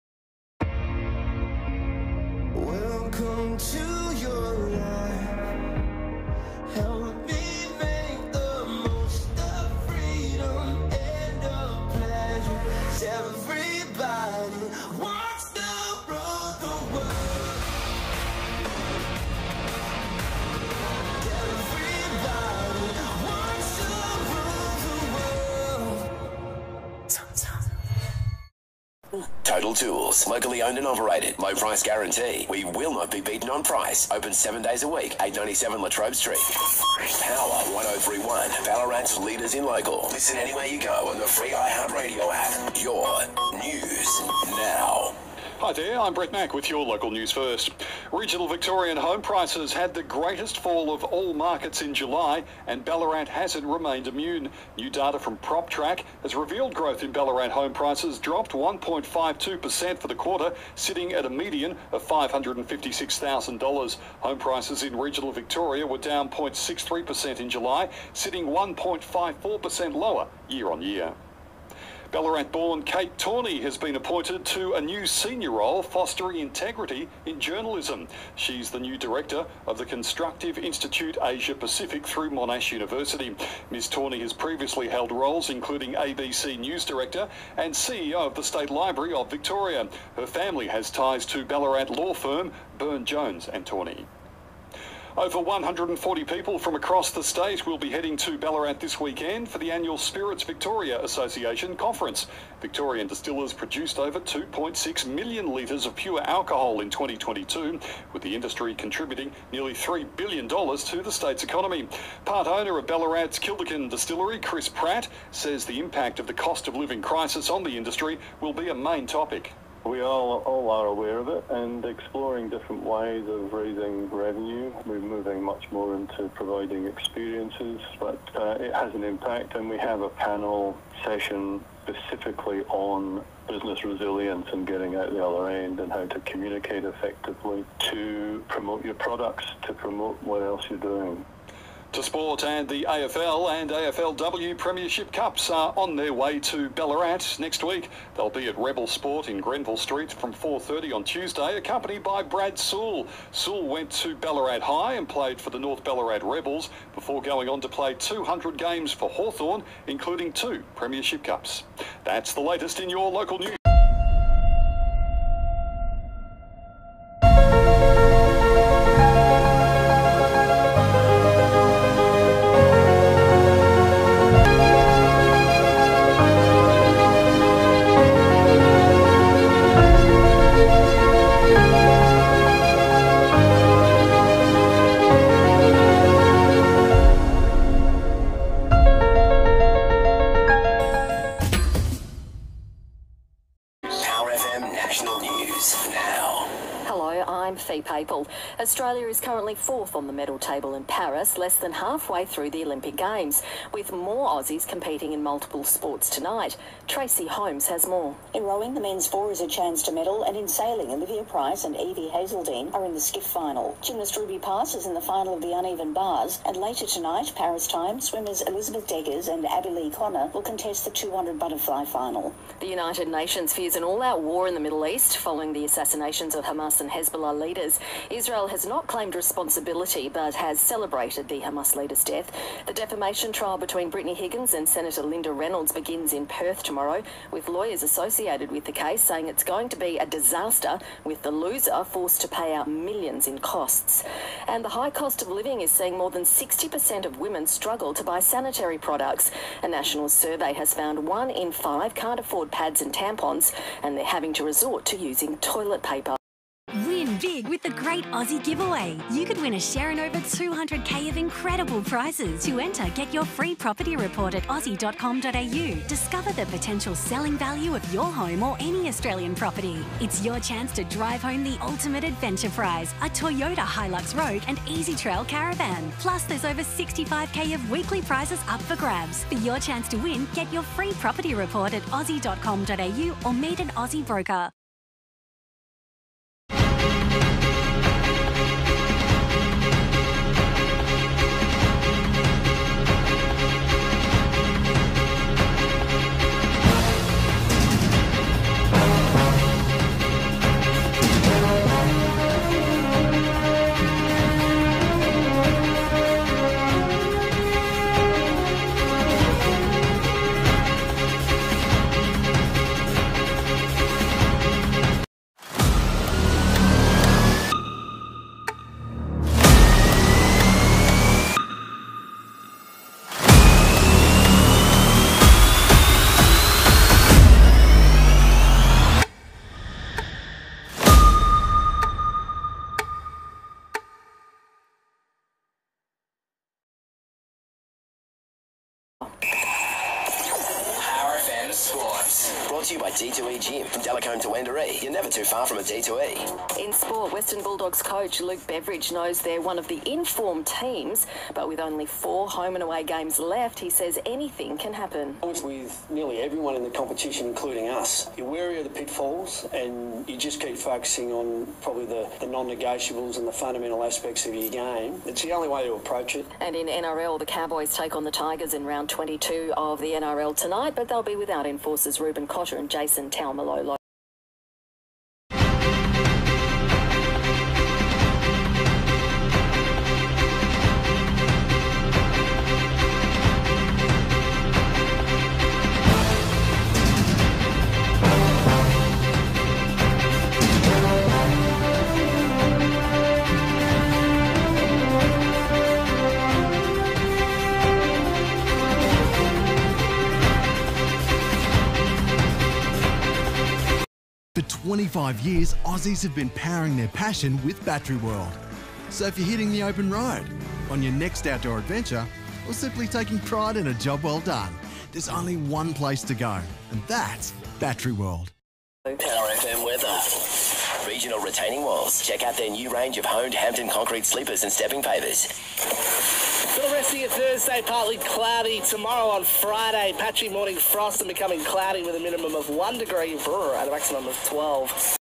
Ballarat Region 103.1PowerFM - 6PM 5News Update, Thursday August 1st 2024
With Brett Mack (Local News First) and Fi Papel (National News and Sport)